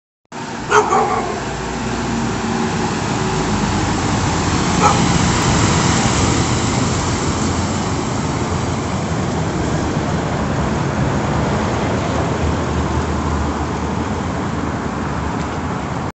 Inrecar Geminis II/M.Benz/LO-916 Buses Ganosa Transportes Tomé
Recorrido 401
18 Septiembre-Tomé-Lirquen-Penco-Concepcion.